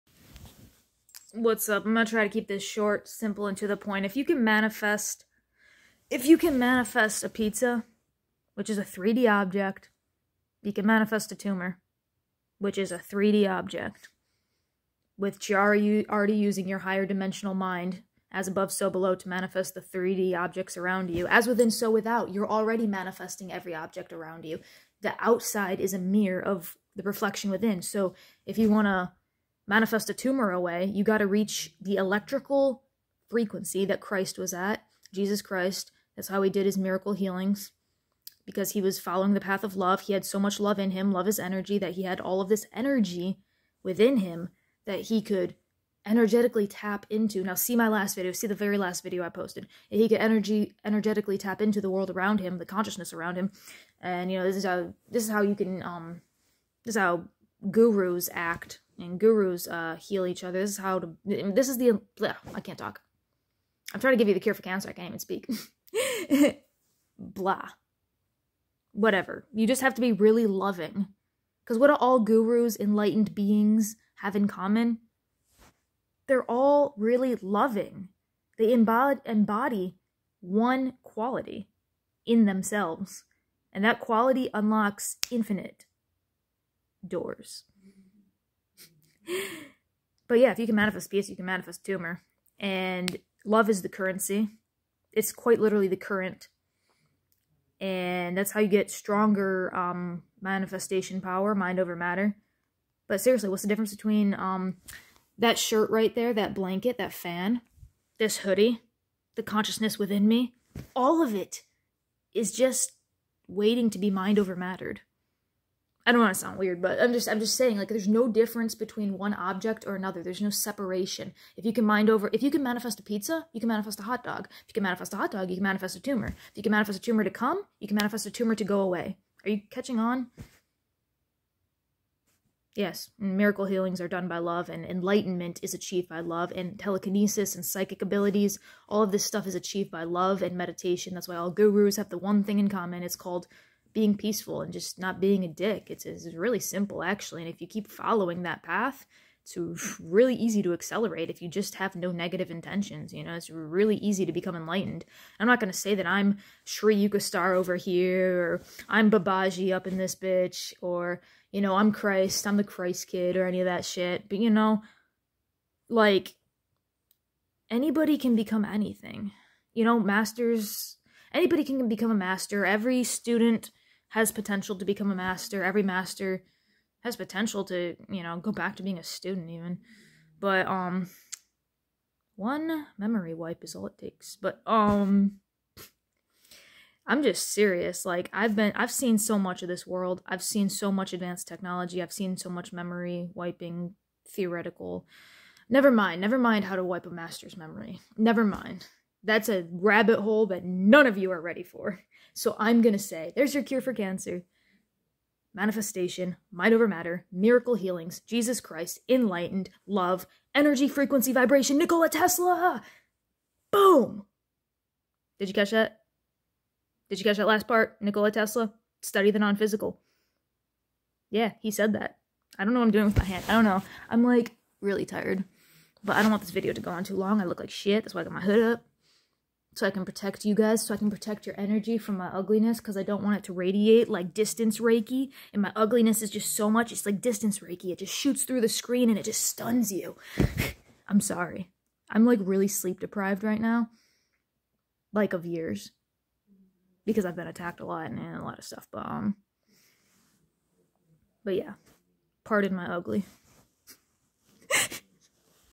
cure for cancer is consciousness activation 👁
ps.pardon my ugly.

p.s ask what manifested the cancer first . perhaps unsettled emotions or even past life trauma.

p.s again JESUS OR BUDDHA COULDNT GET CANCER BECAUSE THEIR SYSTEM IS TOO HEALTHY WITH LOVE AKA CHI/LIFEFORCE ENERGY. enlightenment cures all disease .

dark cannot grow in the field of light. -an old alchemist named me